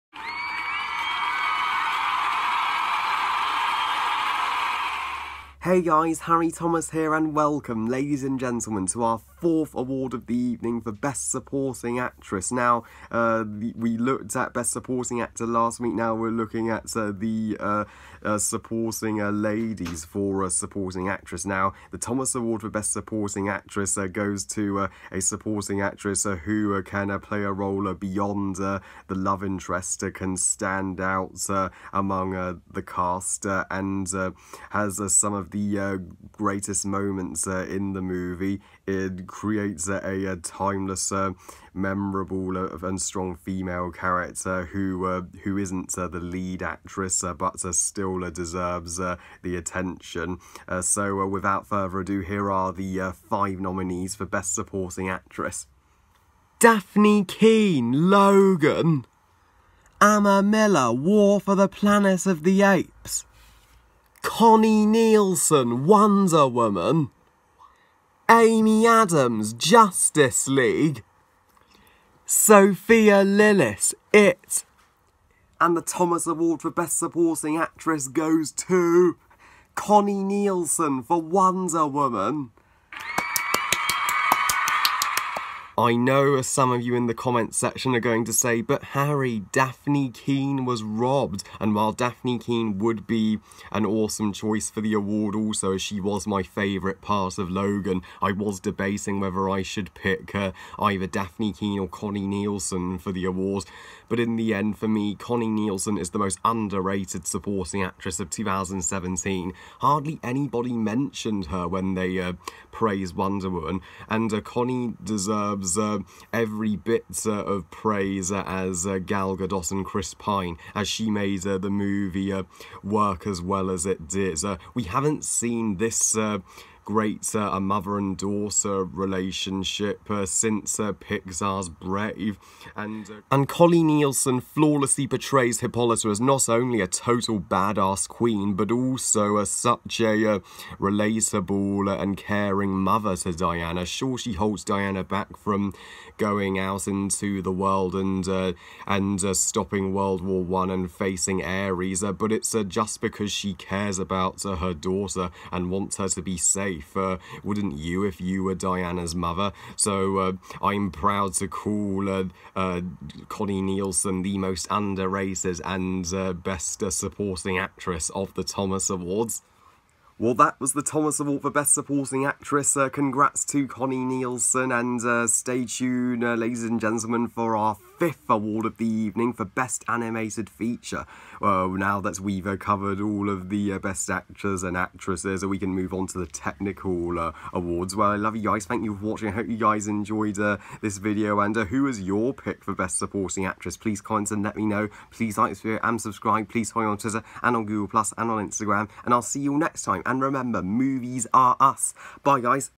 The Thomas Awards 2018 Part 4 Best Supporting Actress
For the 4th Award Of The evening we look at the supporting ladies who deserve as much attention as the lead actress. Who’s your choice for the award out of Dafne Keen, Amiah Miller, Connie Neilson, Amy Adams, Sophia Lillis. Enjoy Guys!